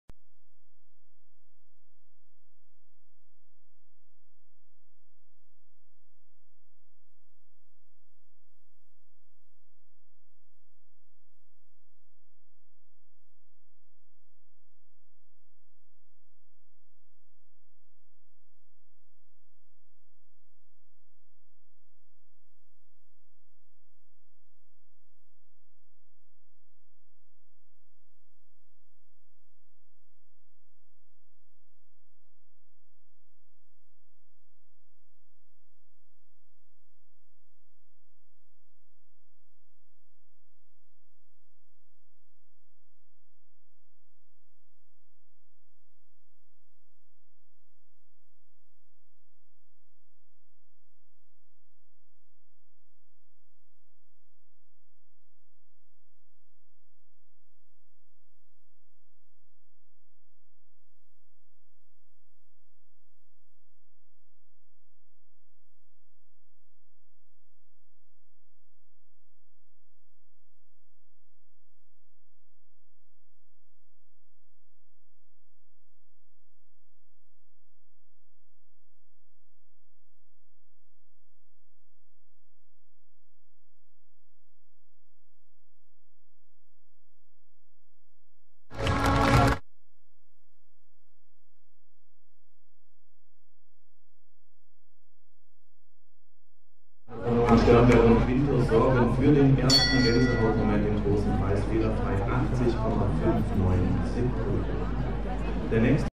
Quintus 145 Alexander von Ungern-Sternberg
23. Freiland Reitturnier des RV Hüttenthal e.V. 2019
01. Springprüfung Kl. S** mit Siegerrunde